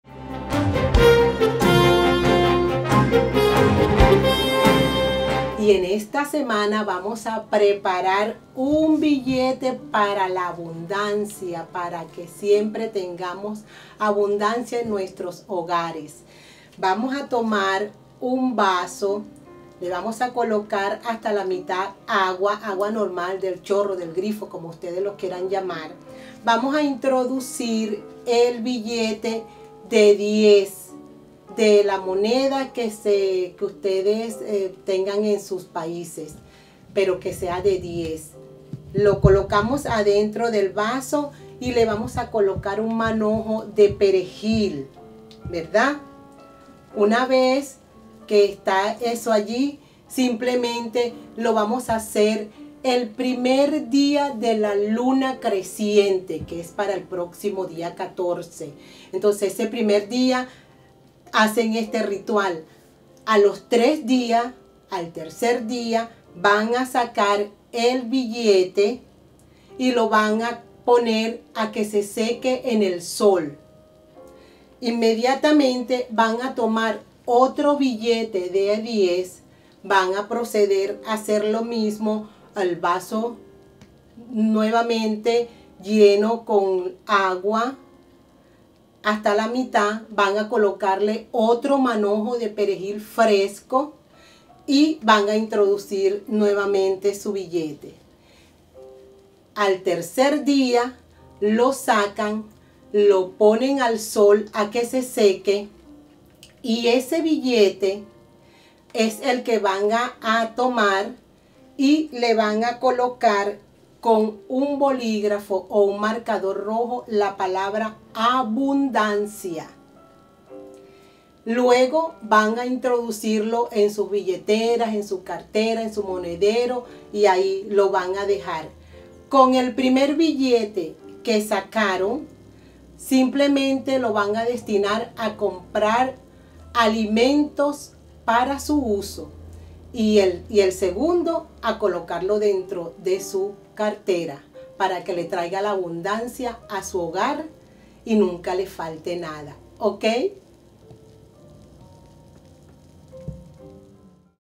Ritual Billete De La Abundancia
Elia Horoscopo Ritual Billete de la Abundancia--desde Nueva York, tu lectura del Tarot en español..SI TE SIENTES CONFUNDIDO, YO TE DOY ALTERNATIVAS PARA EL CAMBIO.